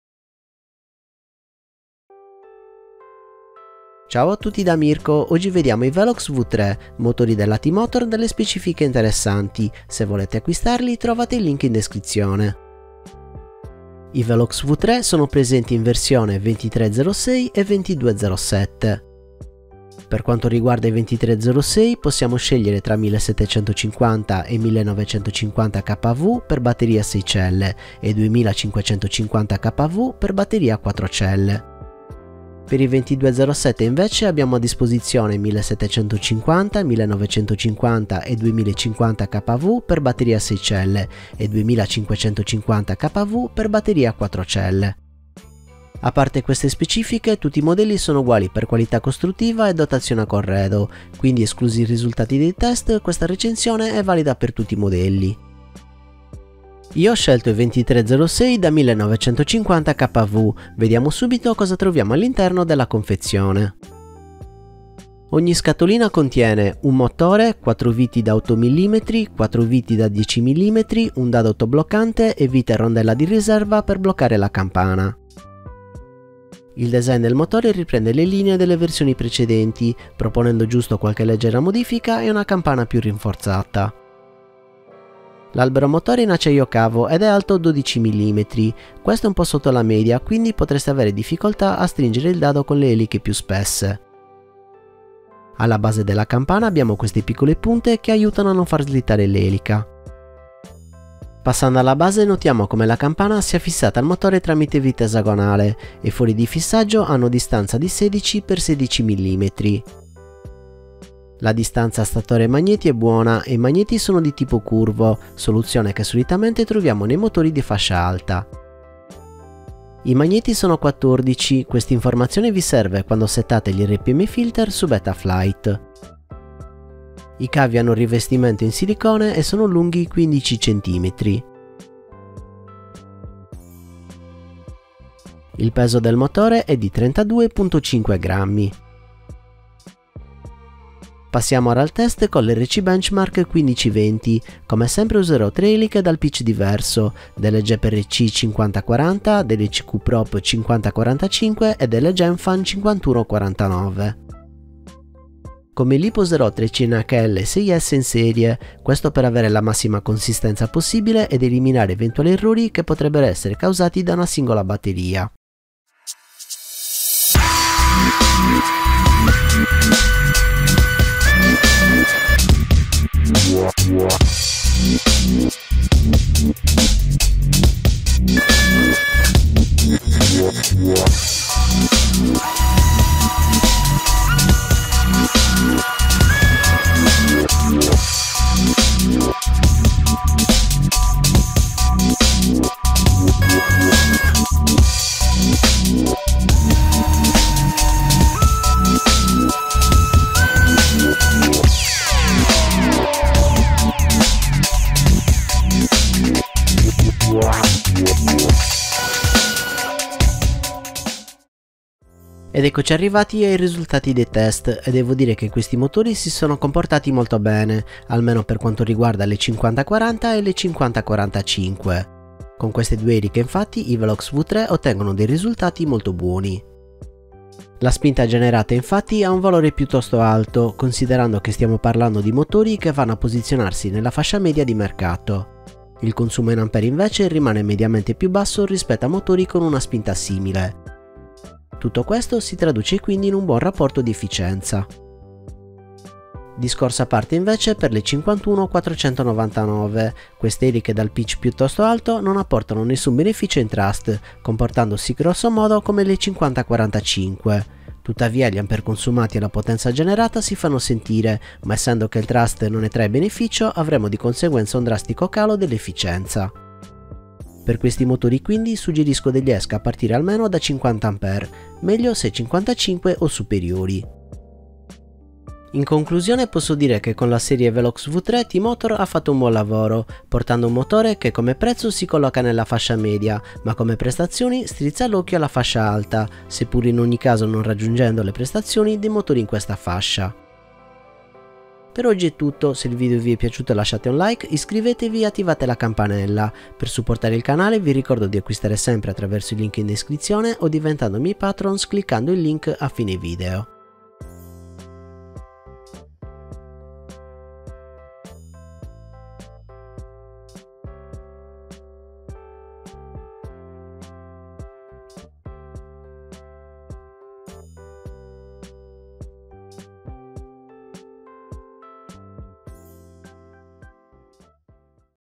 T-Motor Velox V3 - Motor Review (RCB1520)
SEGUIMI LIVE SU TWITCH TUTTI I GIORNI DALLE 21!

▶ Timeline:

00:00 INTRO & UNBOXING
01:16 Specifiche
02:20 Test Motori
03:31 Risultati Test
04:35 Considerazioni Finali